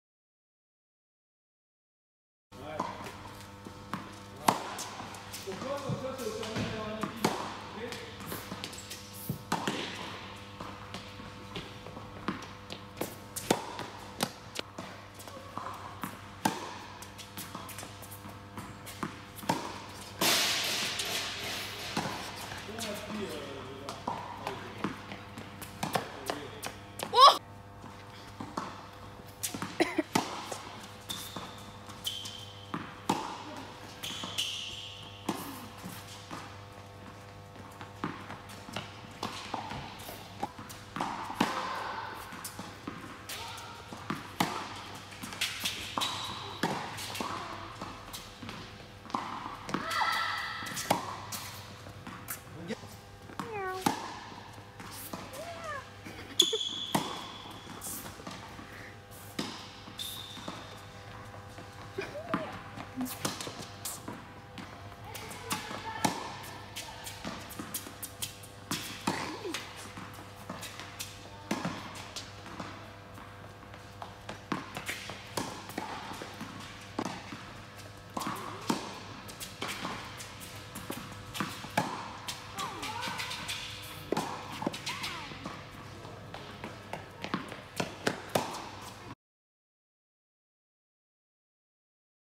Réactions diverses face à un lob au tennis
Réactions diverses face à un lob au tennis avec des jeunes de 10-11 ans. Travail ludique et de gestion de la position dans l'espace.